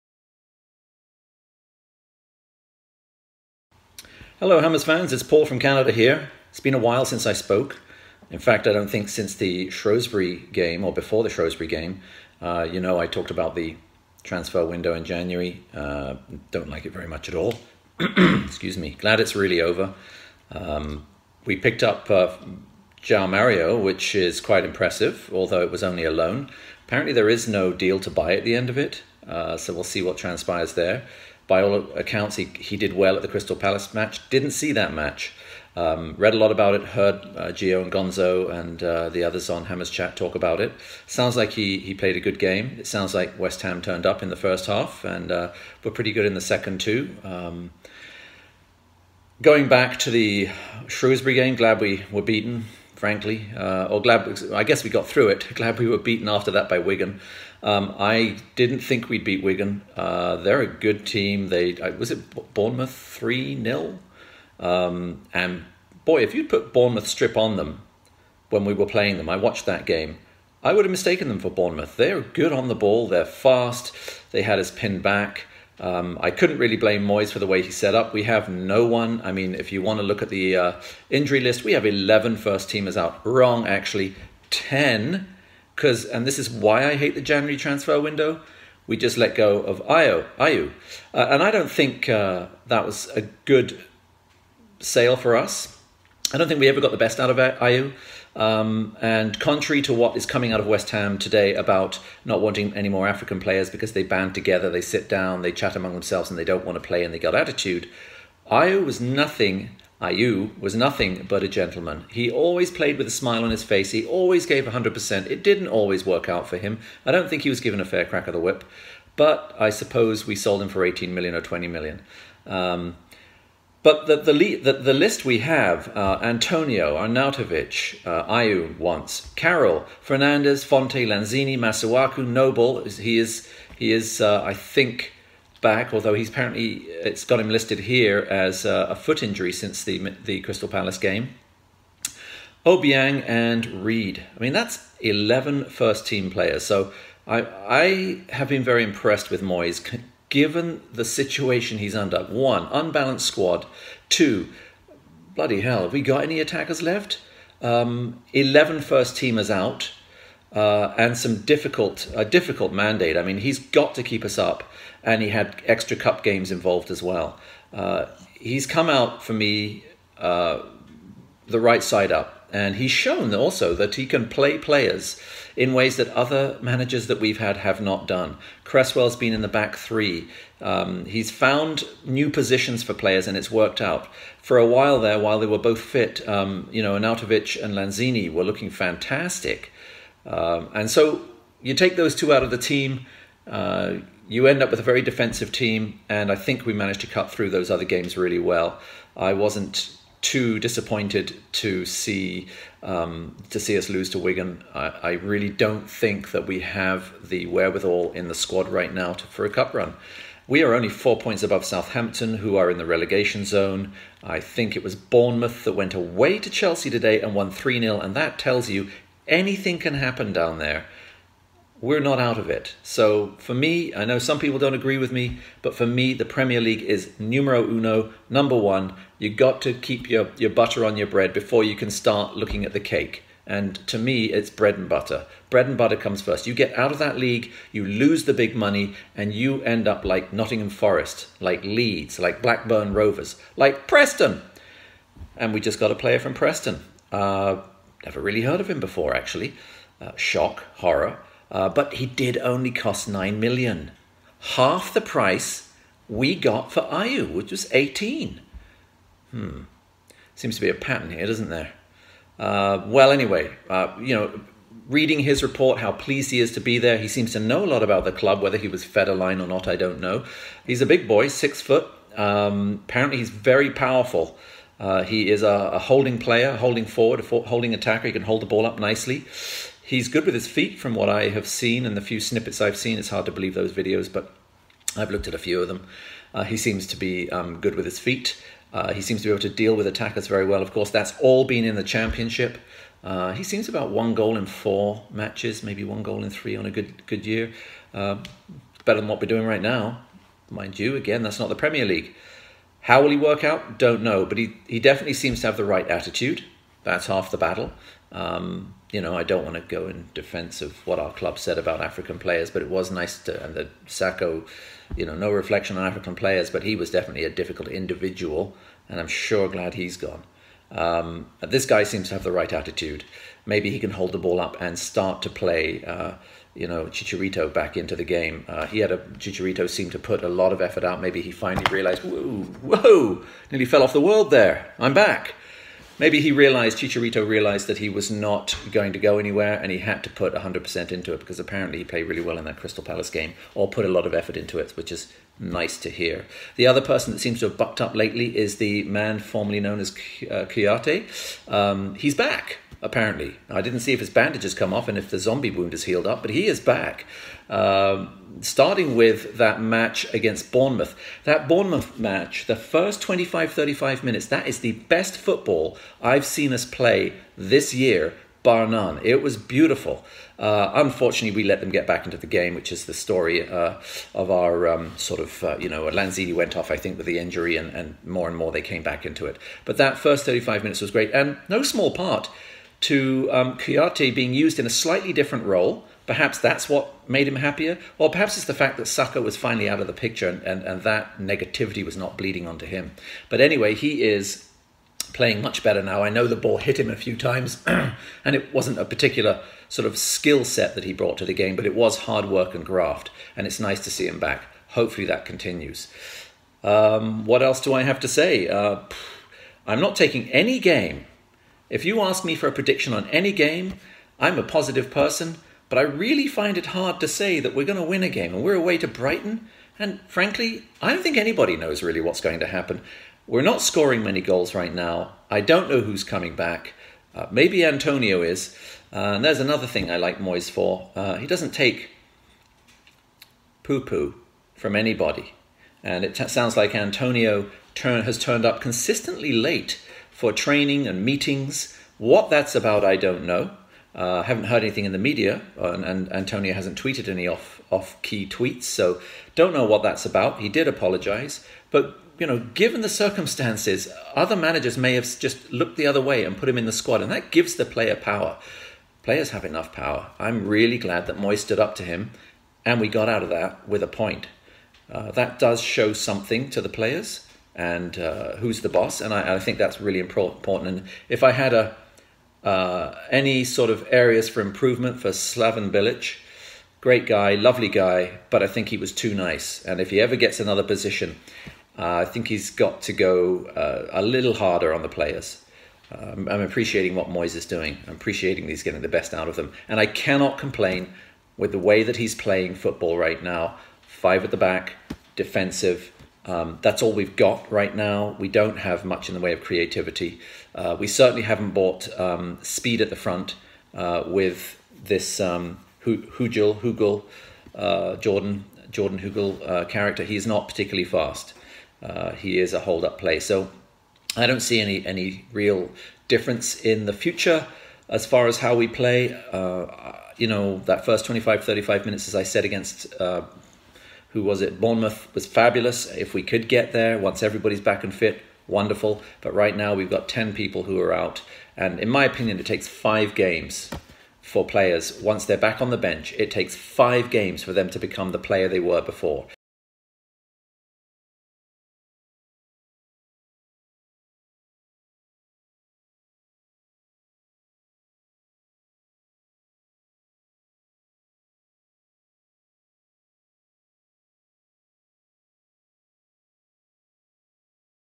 West Ham's Transfer Window | Your Shout | Paul's Thoughts
On this episode Paul records himself talking about our transfer window business 10 minutes after it shut.